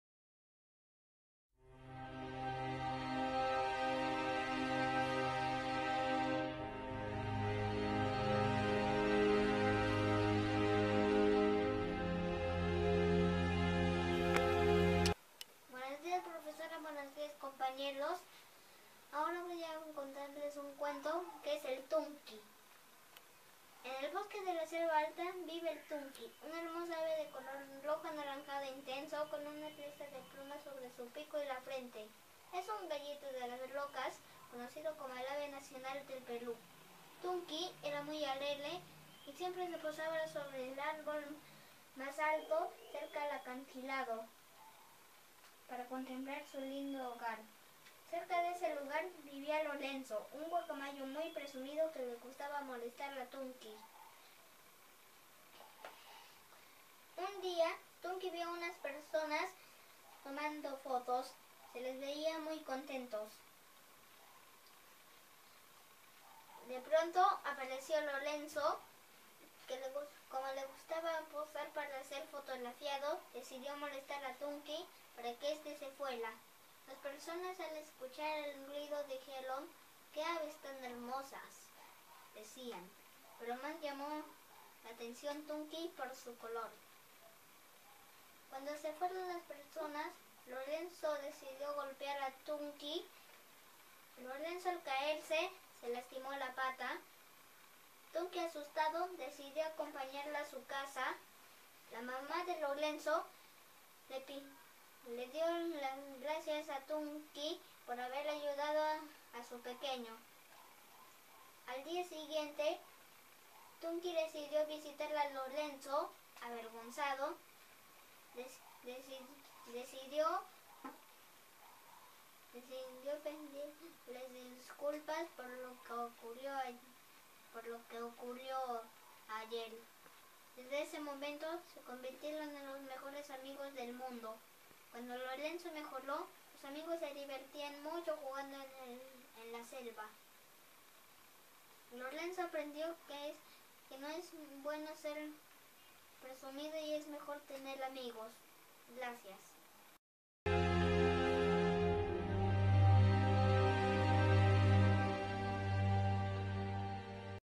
Cuéntame un cuento "TUNQUI"
Un libro abierto es un cerebro que habla, cerrado un amigo que espera, olvidado un alma que perdona, destruido un corazón que llora. Te invito a ver este cuento relatado por Jheremy Mendoza alumno del 3045 a cargo de la Lic. Betty Rojas